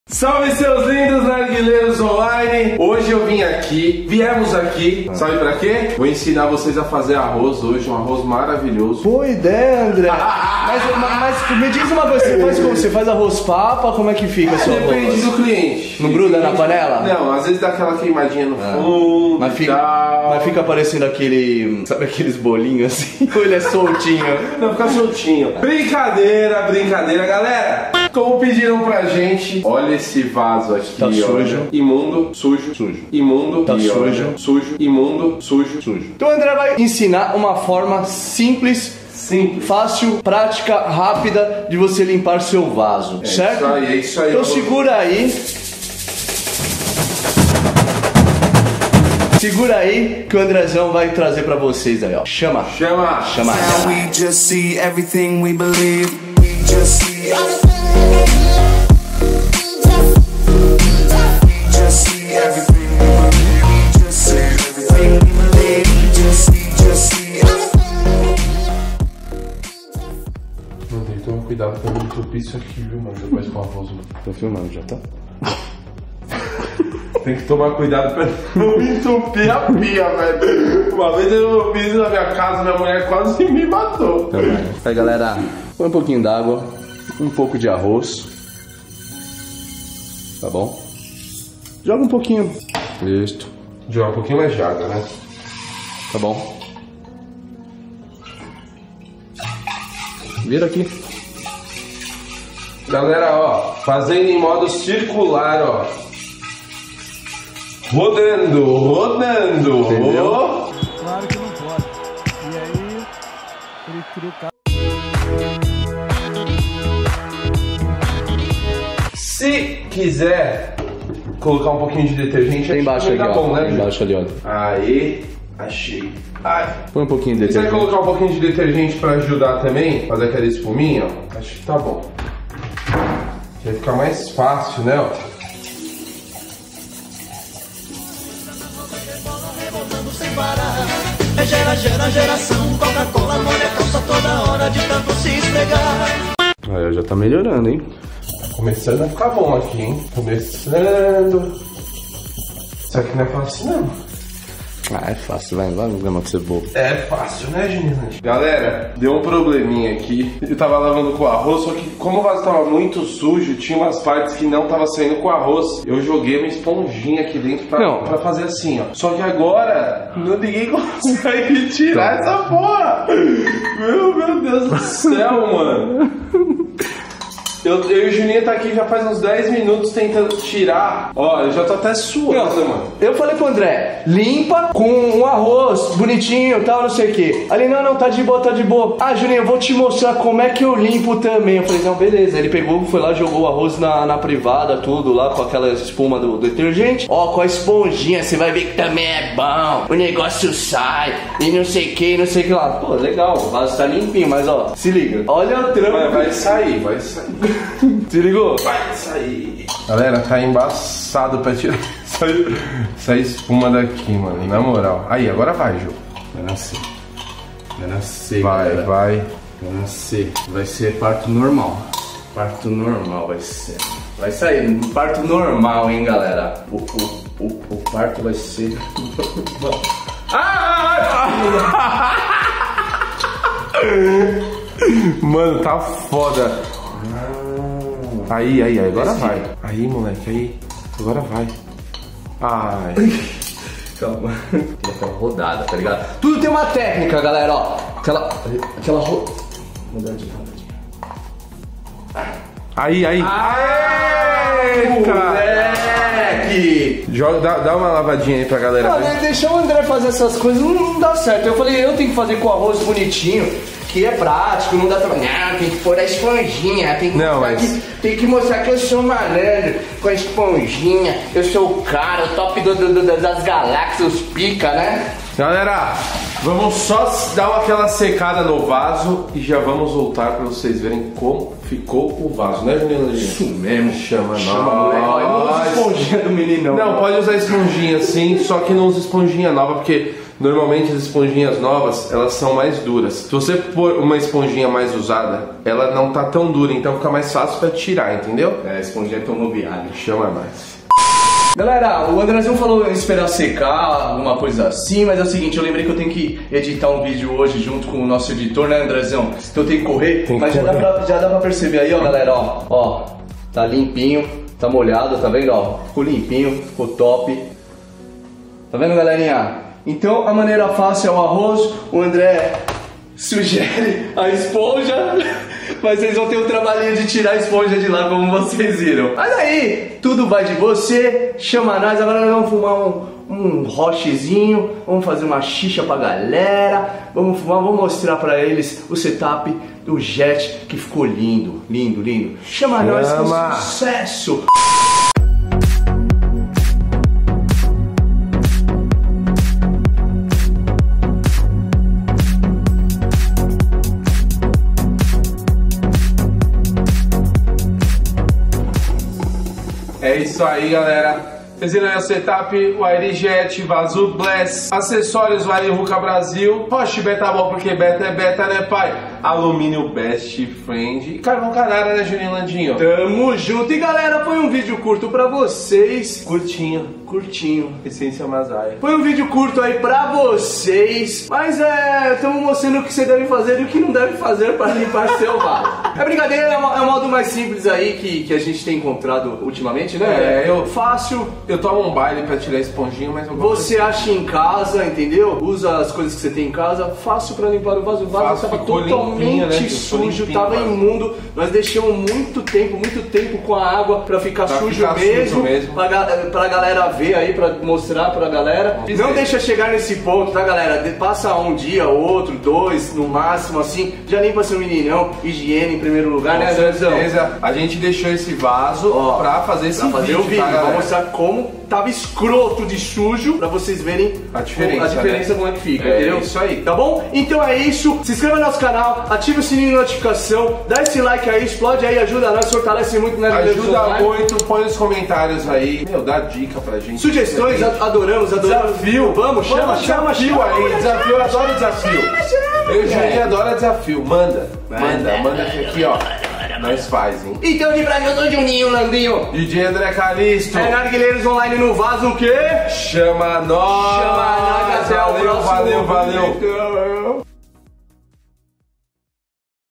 Como limpar vaso de narguile - NARGUILEIROS ONLINE
Mostrando como limpar o vaso de seu Narguile de uma forma fácil
Narguileiros Online com Juninho Landinho e Dj Andre Calixto.